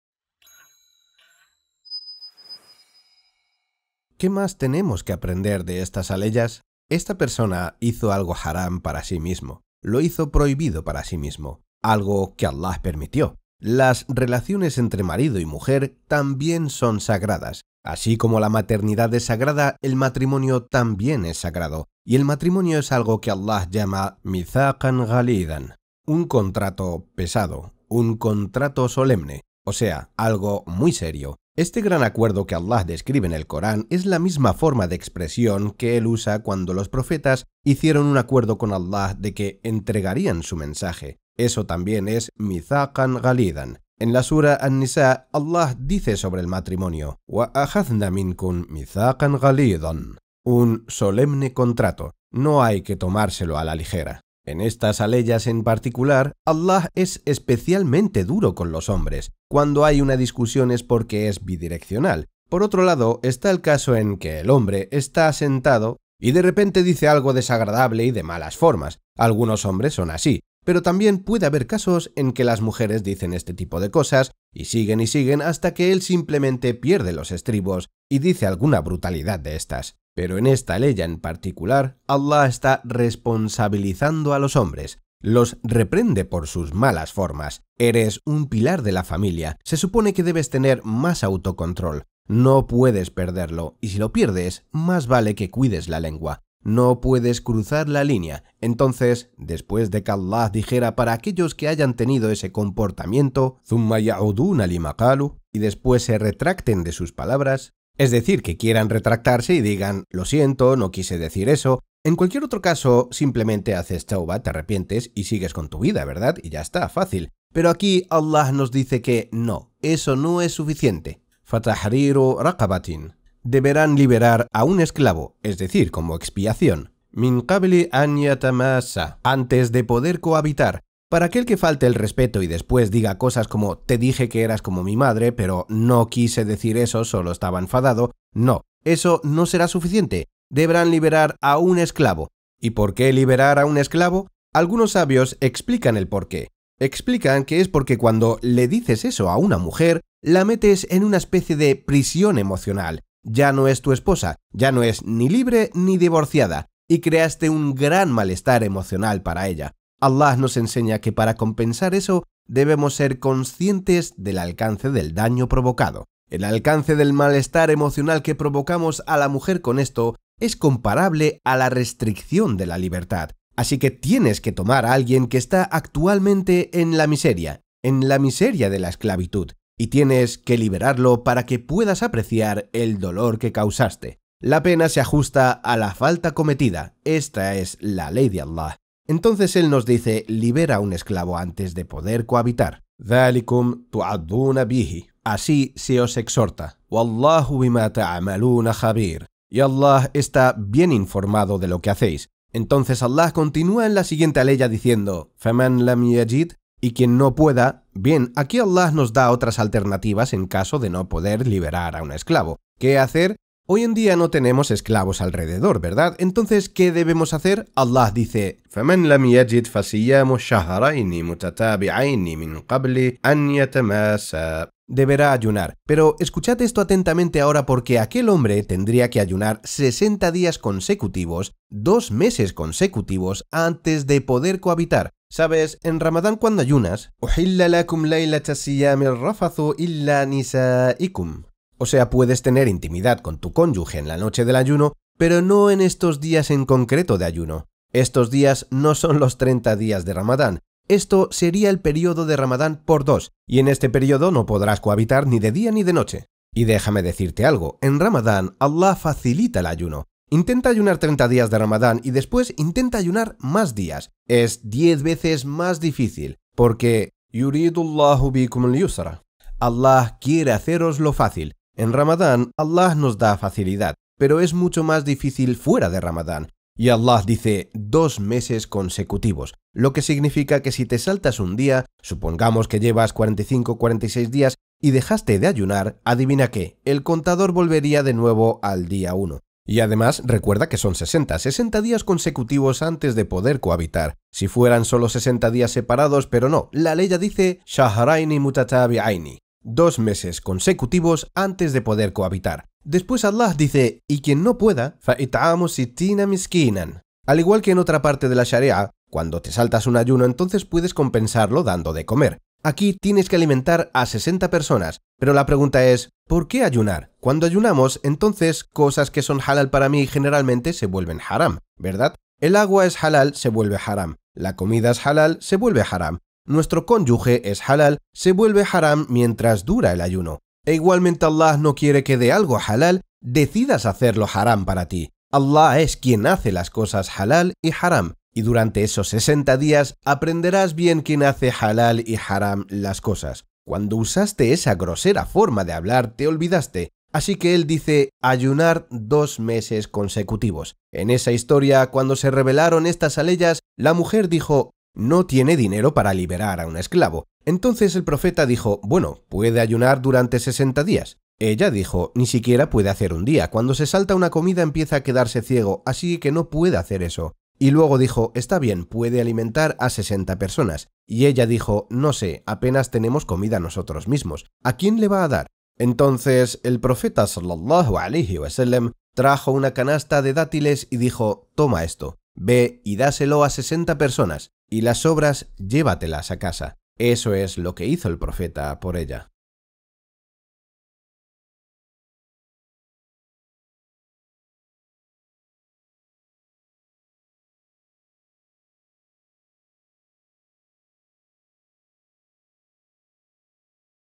Allah escuchó sus quejas Ep.04 - Solo Allah decide qué es Halal o Haram
Aprende Islam en español - Únete a nuestro canal para obtener todas las ventajas:

Videos de FreeQuranEducation en otros idiomas:
🌍 FREEQURANEDUCATION in OTHER LANGUAGES: